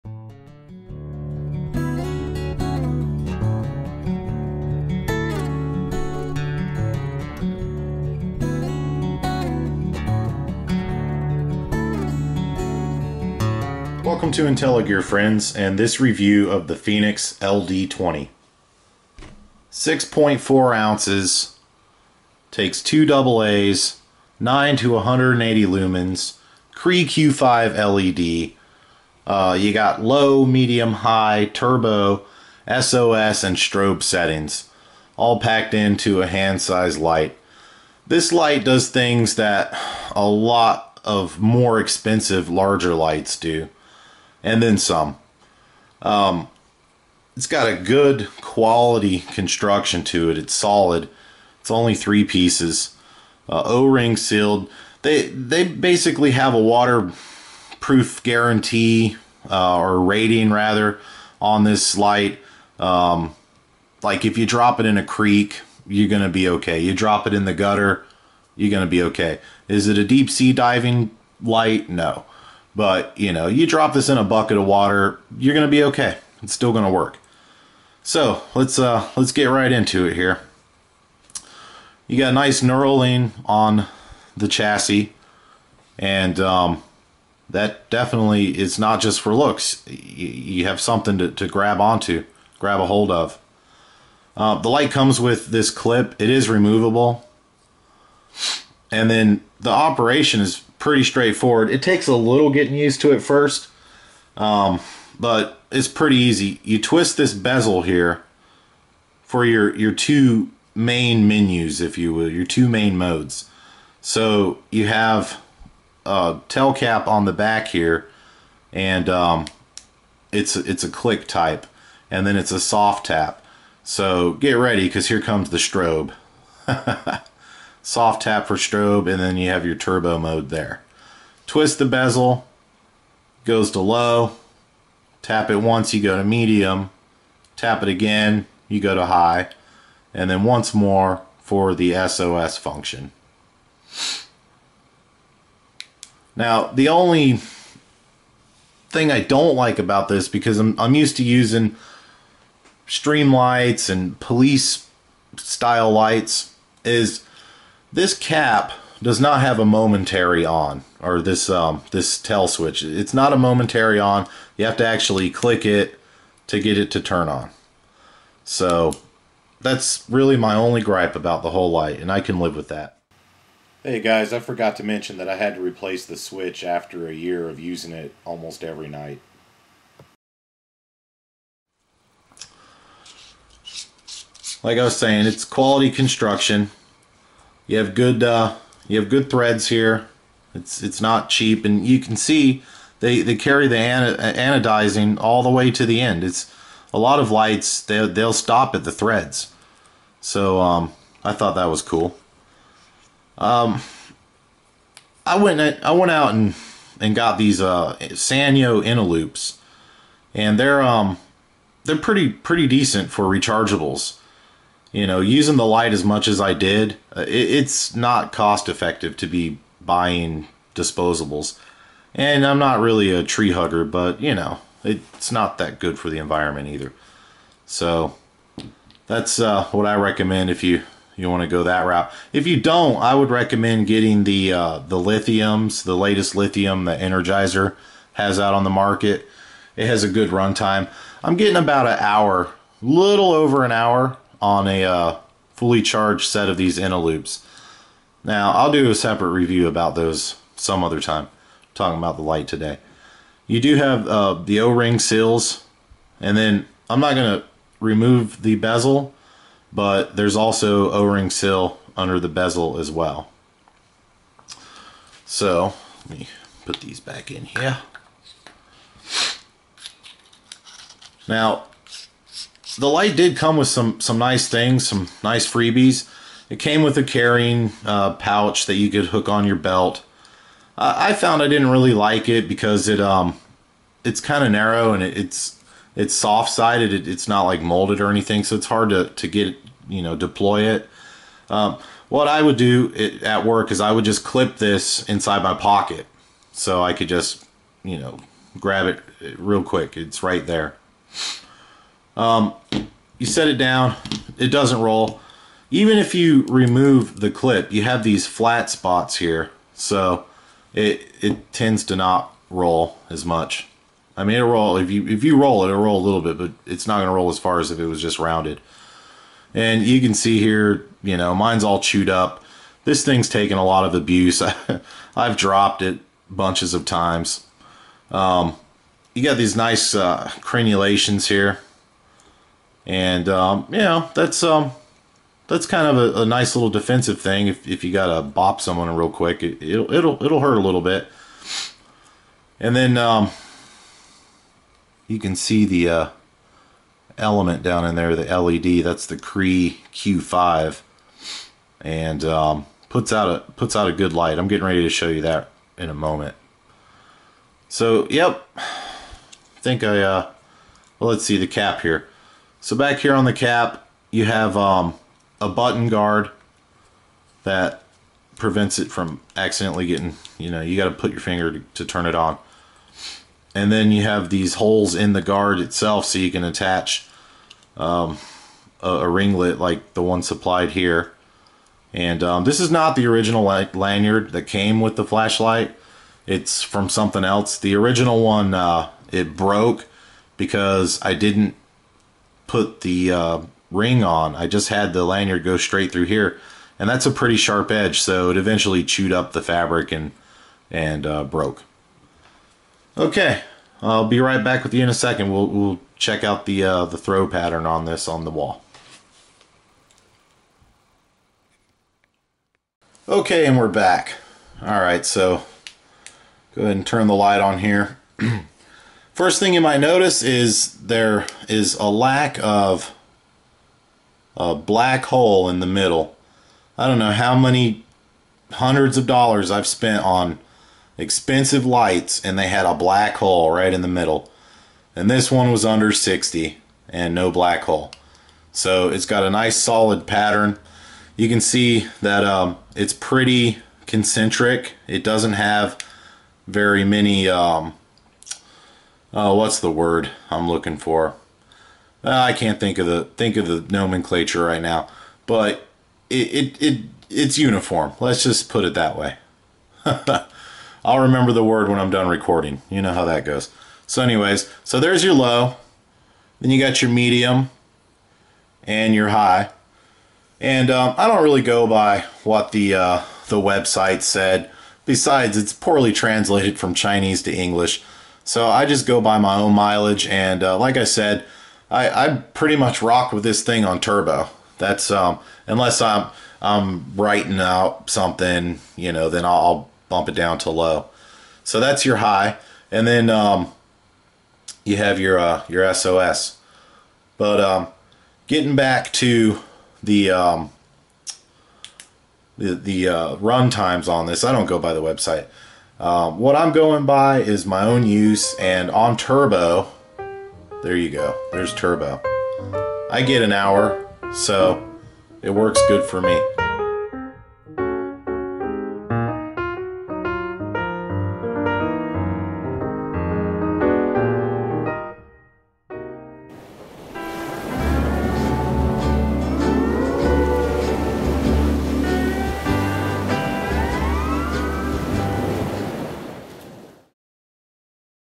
Fenix LD20 - LED Flashlight Review.
Fenix LD20 Review. CR 123 brightness from Two AA batteries! LED Flashlight. This little light is packed with features more commonly found in a standard size more expensive flashlight. Very economical to use thanks to it's two AA power supply when compared to more expensive battery sizes.